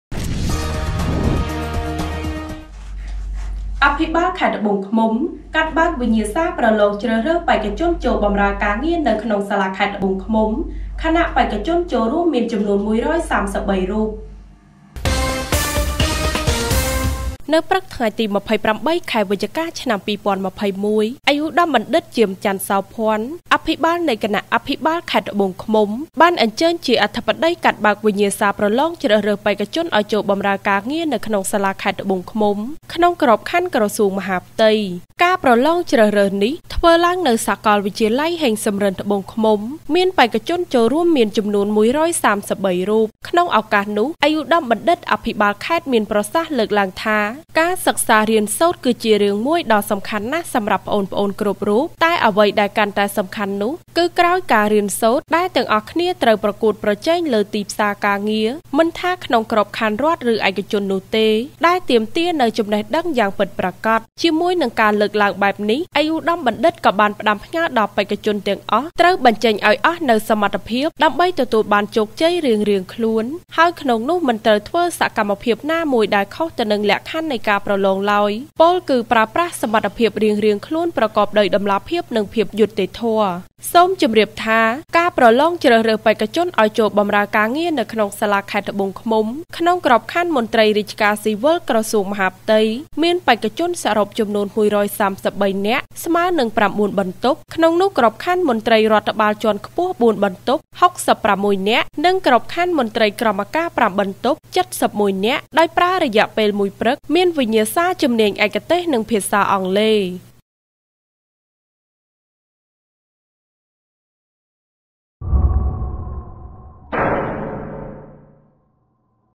អភិបាលខេត្តត្បូងឃ្មុំ កាត់បើកវិញ្ញាសារប្រឡងជ្រើសរើសបេក្ខជនឲ្យចូលបម្រើការងារនៅក្នុងសាលាខេត្ត
អភិបាលខេត្តត្បូងឃ្មុំ កាត់បើកវិញ្ញាសារប្រឡងជ្រើសរើសបេក្ខជនឲ្យចូលបម្រើការងារនៅក្នុងសាលាខេត្តត្បូងឃ្មុំ ខណៈបេក្ខជនចូលរួមមានចំនួន១៣៣រូប